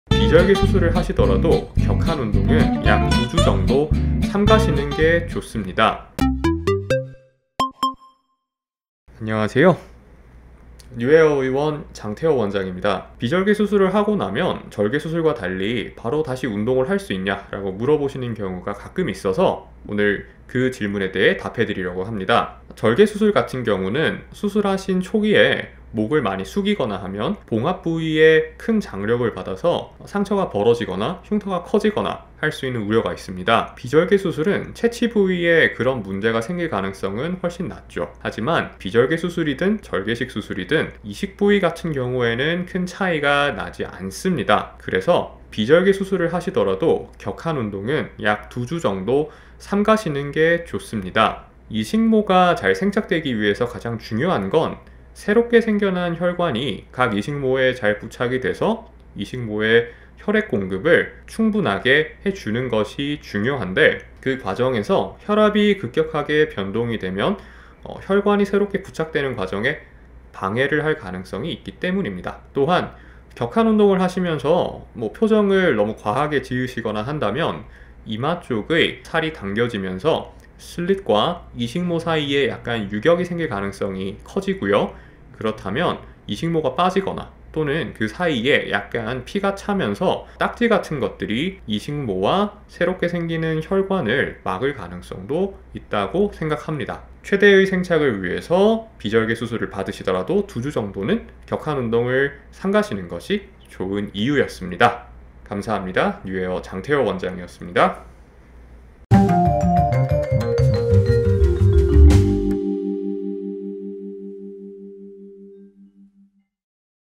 모발이식 비절개로 수술하면 바로 운동해도 돼요?ㅣ뉴헤어 [모발이식][모발이식 후기]
[모발이식][모발이식 후기]

@newhair_pjt 장태호 모발이식 닥터

비절개로 수술하면 바로 운동해도 될까요?
비절개 모발이식에 대한 모든 것!
뉴헤어 장닥터가 설명해드릴게요😉